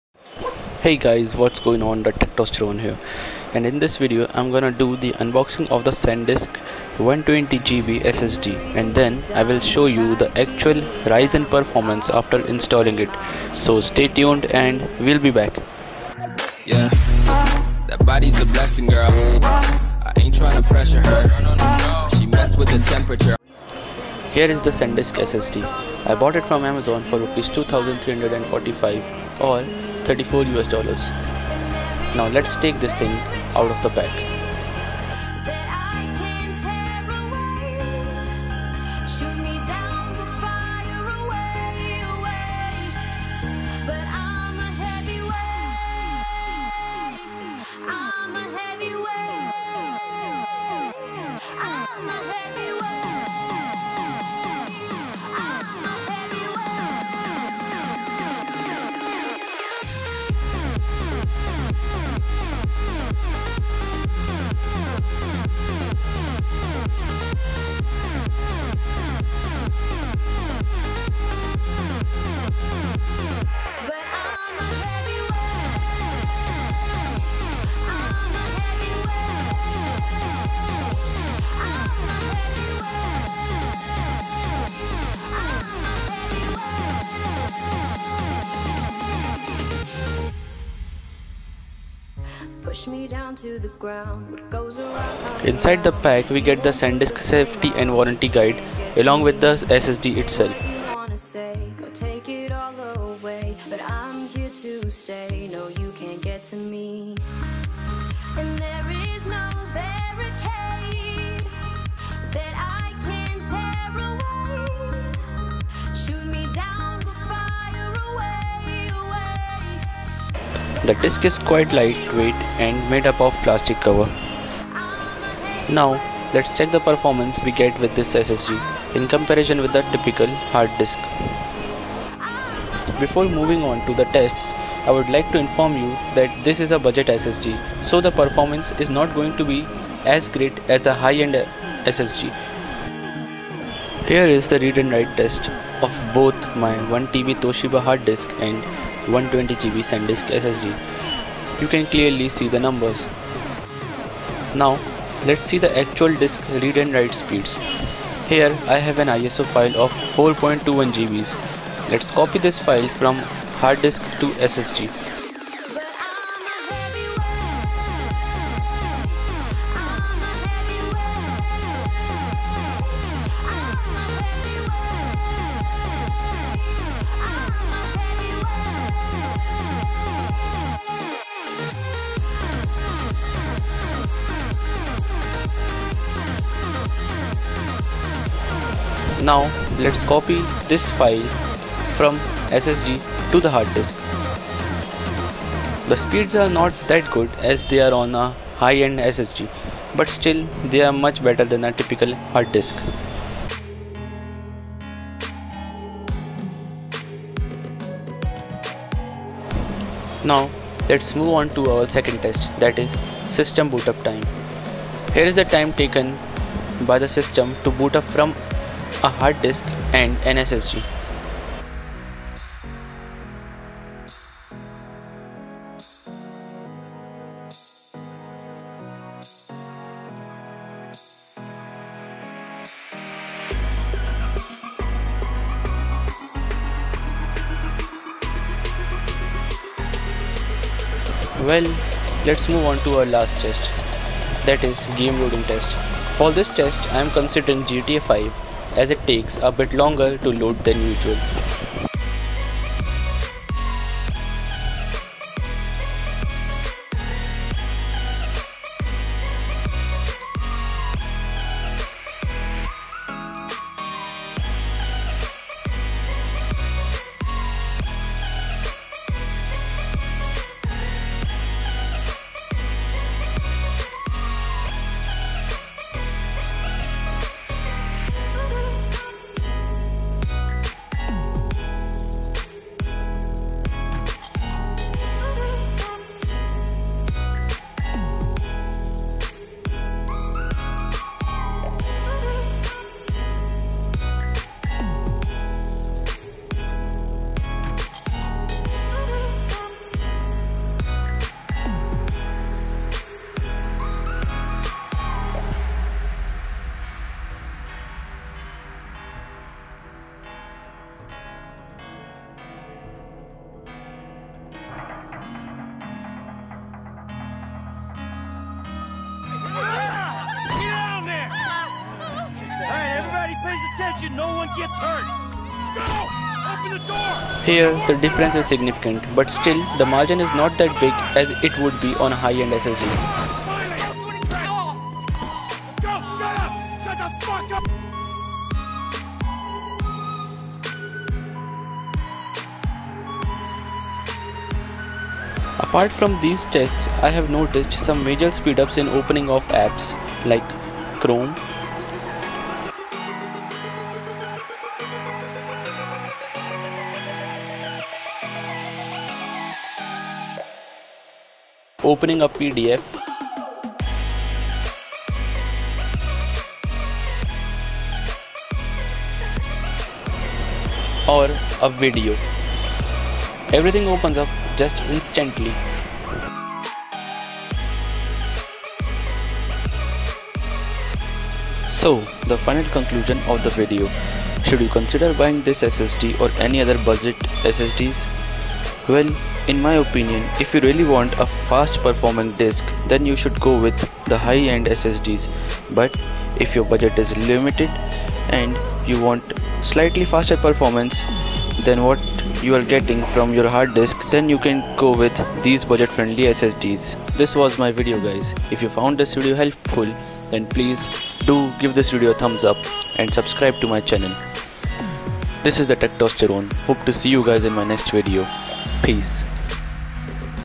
SanDisk SSD Plus 120GB REVIEW & UNBOXING With TESTS | Budget SSDs Worth Buying?
In this video I have the unboxing and REVIEW of the SanDisk SSD Plus 120GB with System BOOTUP TEST, DISK READ & WRITE TEST and GAME LOADING TEST. I have compared the SSD with my Toshiba 1TB Hard Disk Drive.